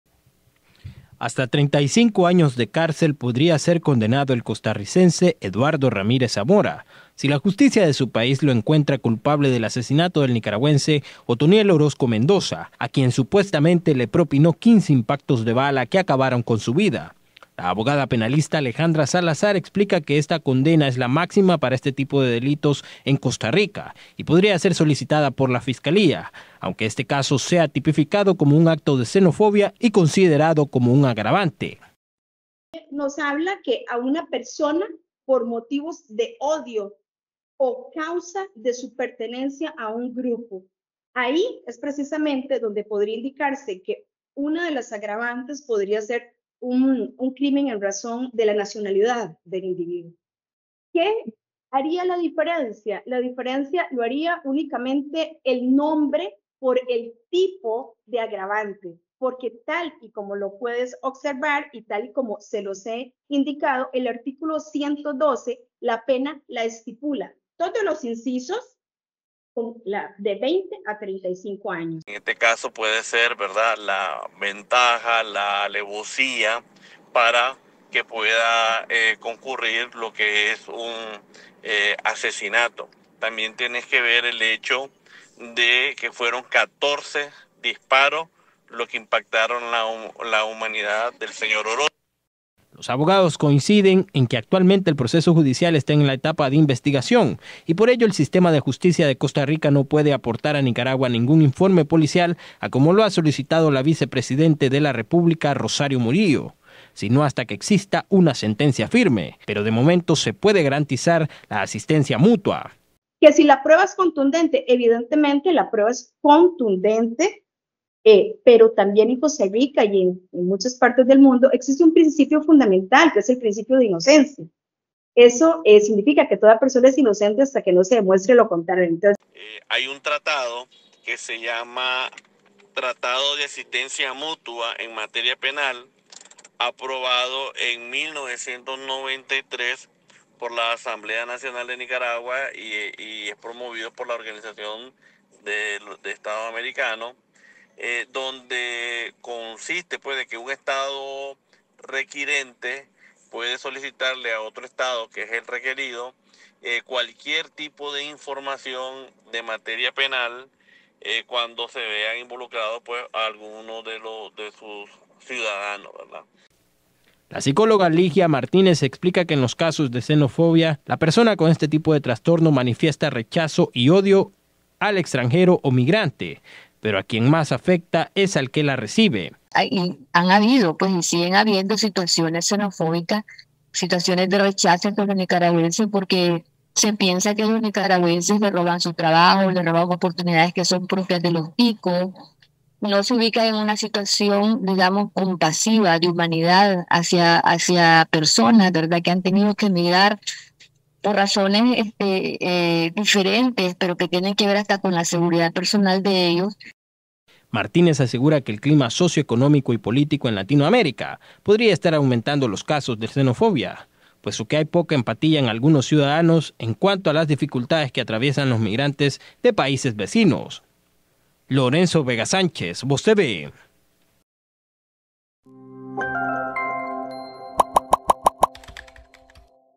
35 años de cárcel podría ser la condena del tico que privó de la vida a nicaragüense
La abogada penalista Alejandra Salazar explica que esta condena es la máxima para este tipo de delito en Costa Rica y podría ser solicitada por la fiscalía, aunque este caso sea tipificado como un acto de xenofobia y considerado como una agravante.